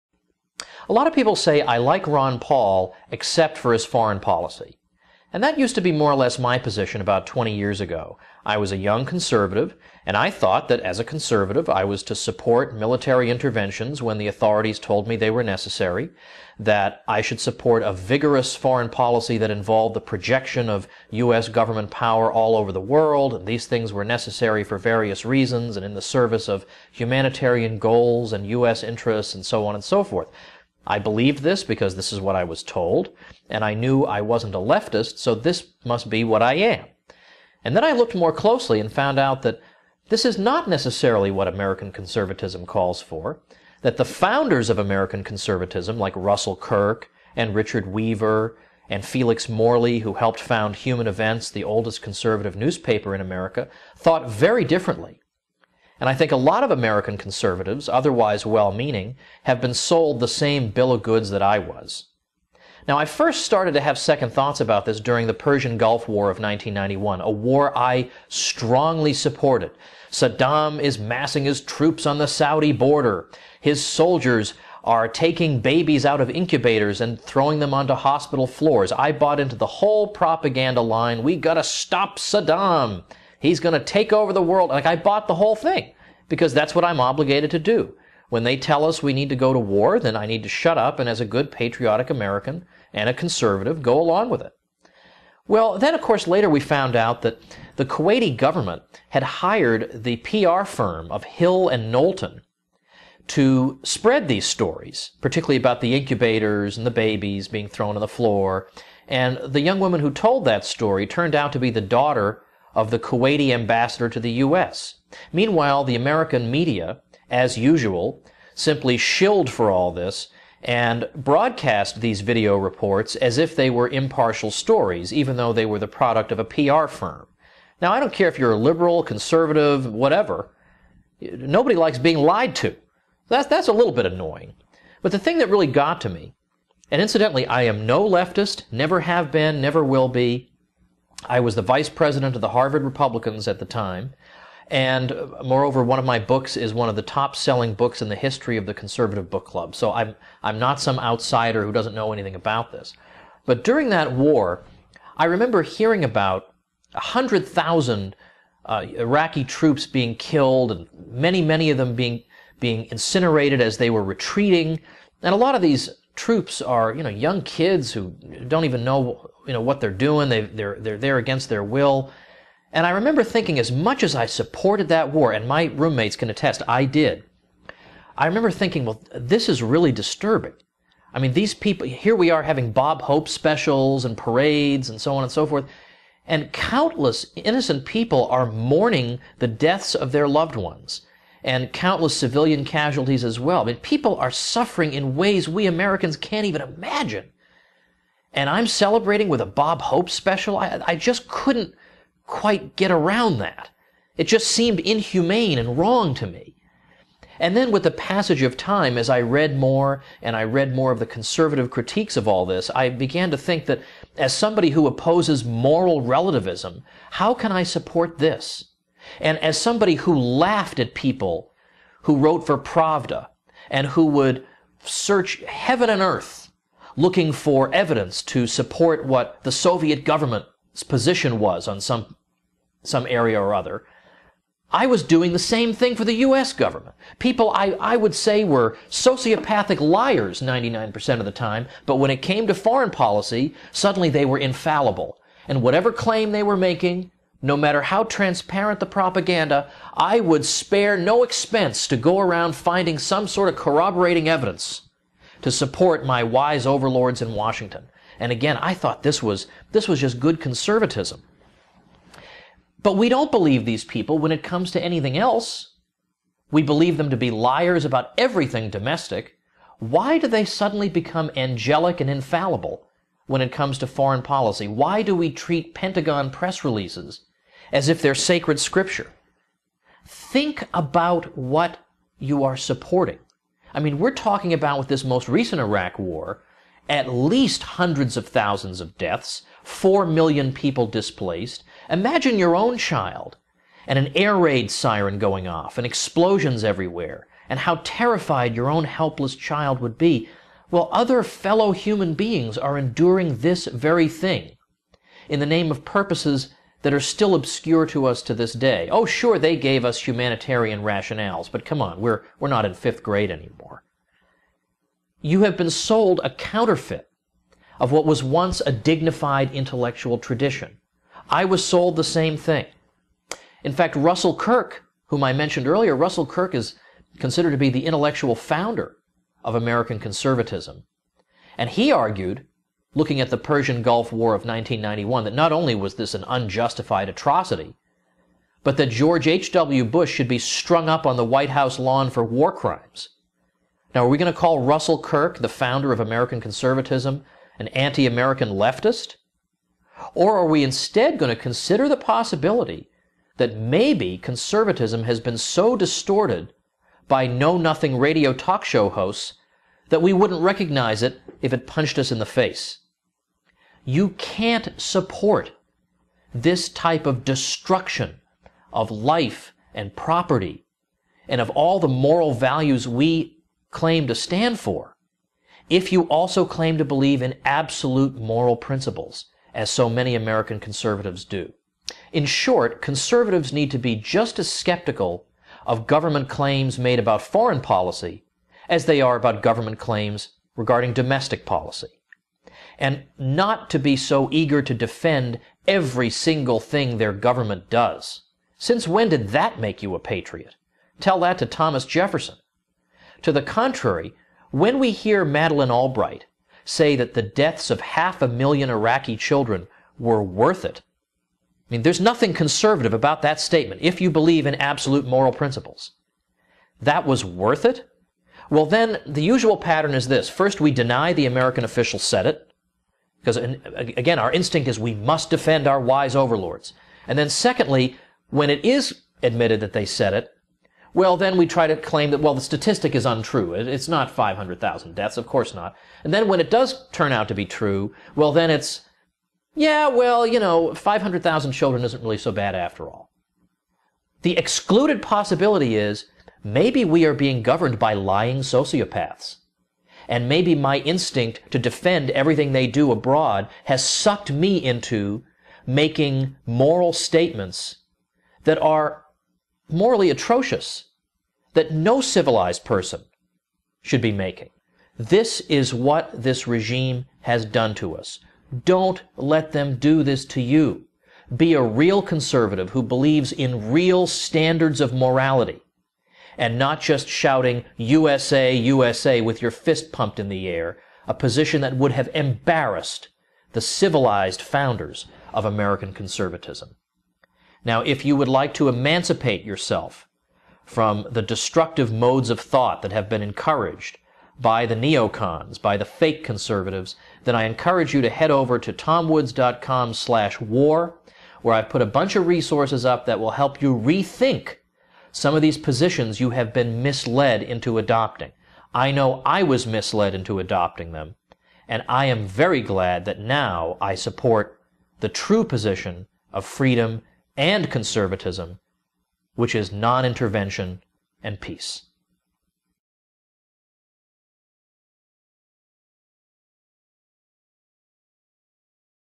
War, Ron Paul, and Conservatism
Tom Woods comments on those who say, "I like Ron Paul except for his foreign policy."